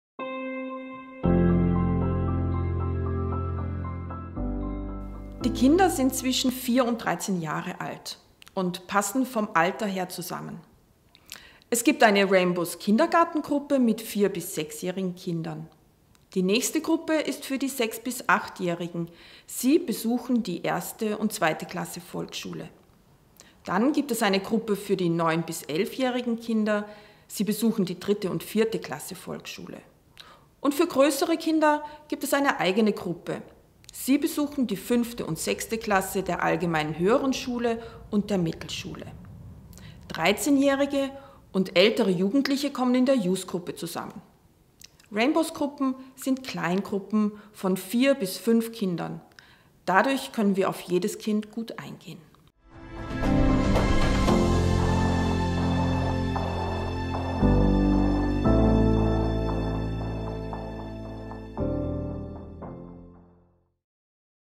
Wie alt sind die Kinder in einer RAINBOWS Gruppe?
In welchem Alter die Kinder in den RAINBOWS-Gruppen sind, fasst unsere Bereichsleiterin aus Oberösterreich zusammen.